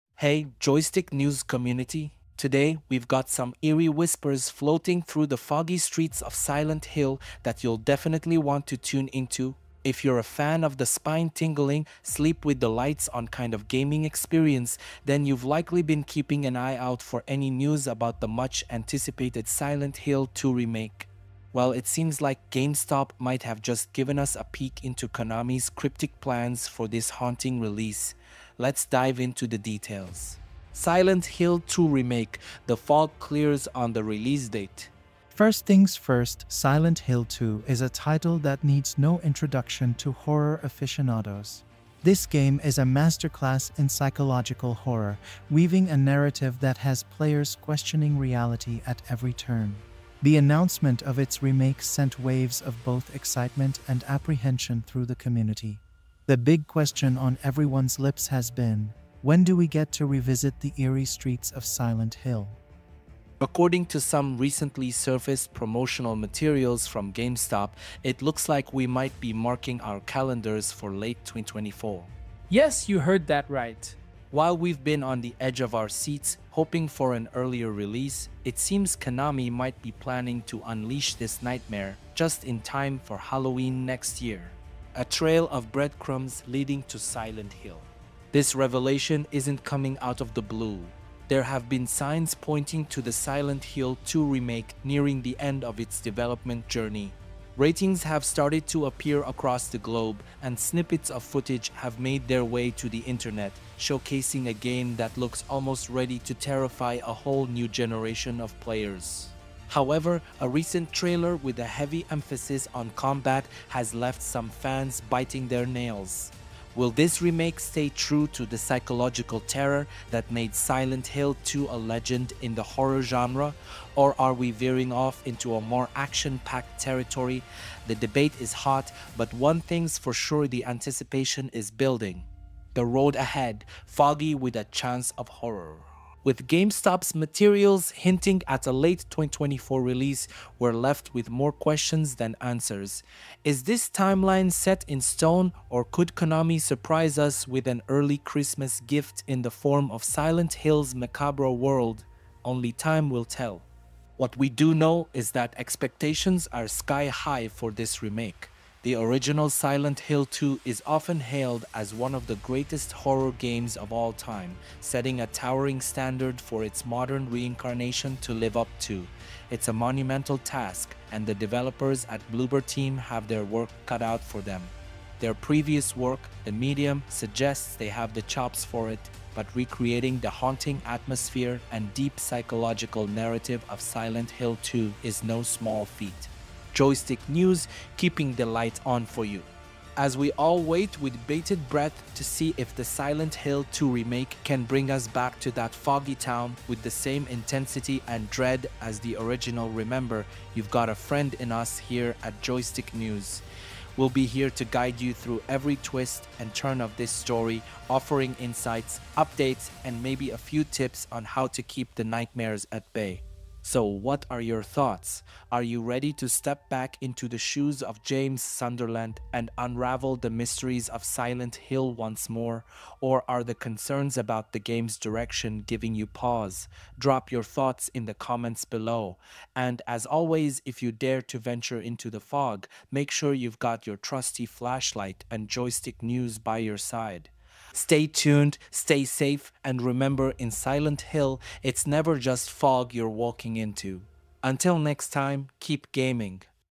Silent Hill 2 Remake Unveiled: GameStop Leaks Release Date & Fans Debate Combat Focus!
Hey, Joystick News community! 🎮 Today, we're tiptoeing through the fog-laden mysteries surrounding the highly anticipated Silent Hill 2 remake. If heart-pounding, psychological horror is your game, you're in the right place. We've got the latest on Konami's plans for this chilling release, sparked by recent whispers and a GameStop revelation that's set the community abuzz.

👻 Silent Hill 2 Remake: The Awaited Return to Terror:
Silent Hill 2, a cornerstone of psychological horror, is making a comeback, and the anticipation is palpable. But when can we expect to tread the haunted streets of Silent Hill again? A leaked timeline from GameStop suggests a spine-chilling late 2024 release. Is this the horror feast we've been waiting for, or is there more to the story?

🕵️ A Mystery Wrapped in Fog:
With global ratings surfacing and tantalizing footage leaks, the Silent Hill 2 remake seems on the cusp of breaking its eerie silence. Yet, a trailer hinting at a combat-heavy approach has the community divided. Can the remake preserve the original's soul-shaking fear, or will it morph into something unrecognizably action-focused?

🔍 The Expectation Game:
Bloober Team, known for The Medium, faces the Herculean task of living up to Silent Hill 2's legacy. How will they balance the game's iconic atmospheric dread with modern gameplay expectations? And with GameStop hinting at a Halloween 2024 release, the countdown to terror (or disappointment) begins.

🎙️ Joystick News: Your Beacon in the Fog:
Stay locked to Joystick News for the latest updates, insights, and survival tips for the impending horror masterpiece. Will the Silent Hill 2 remake lead us into a new era of psychological terror, or are we wandering into a fog of uncertainty? Share your thoughts, fears, and speculations in the comments below.